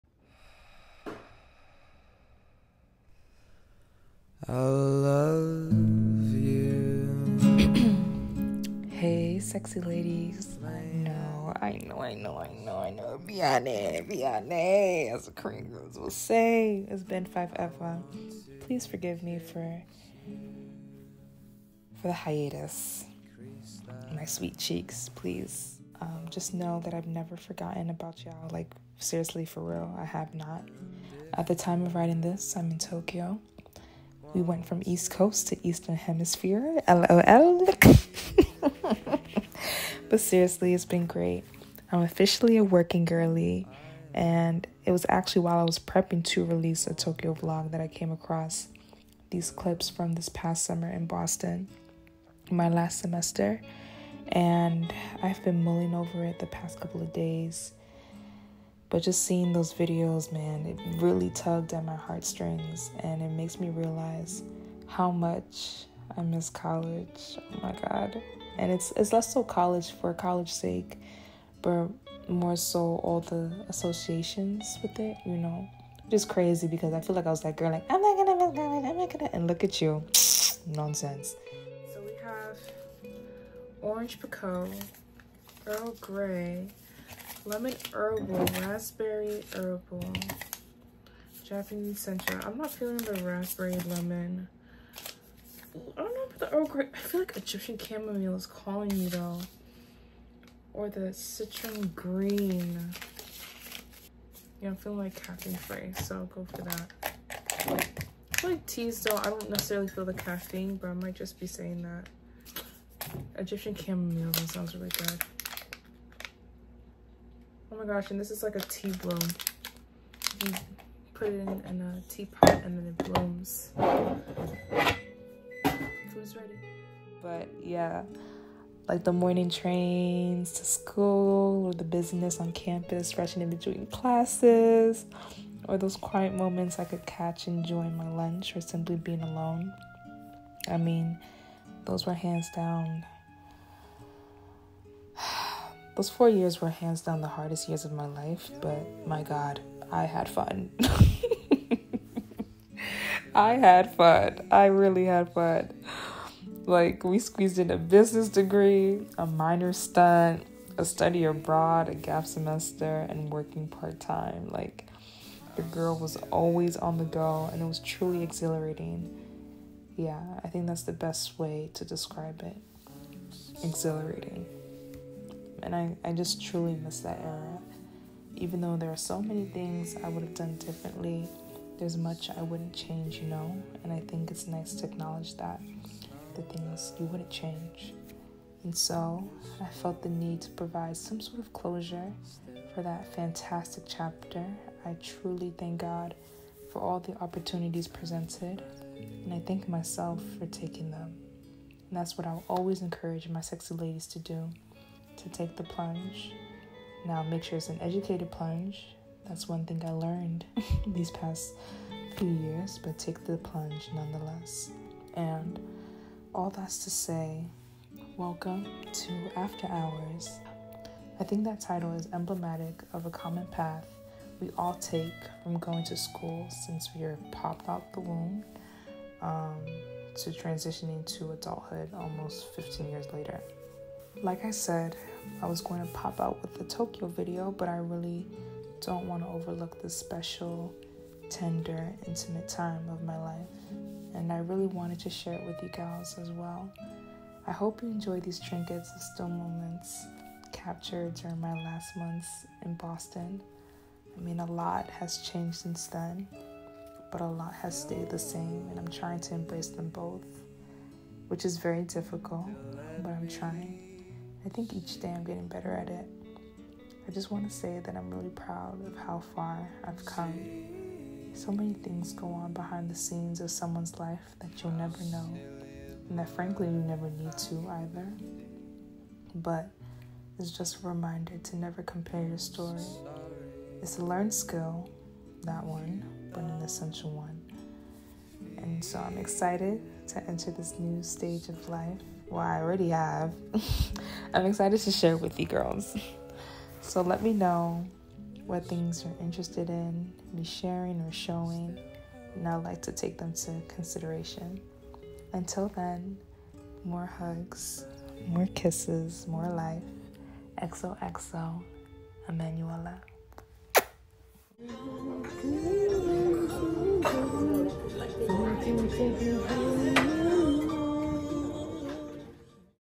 life post grad + moving to tokyo | after hours 001
hey sexy ladies—it's been a super hot minute. so happy to be back and running again. these last couple months...chile. really threw your shawty for a loop, but we're here and we're thriving more than ever. before jumping cold water into my life now in tokyo, i wanted to provide some closure for the significant part of my life before that. i mean, these last 4 years have been all and everything at once. but i'm deeply grateful for all the lessons and experiences along the way. it's truly prepared me for where I am today and where I will be down the road. all this to say, i hope you enjoy these still moments from my past summer in boston. thinking of her. love y'all—xoxo emm.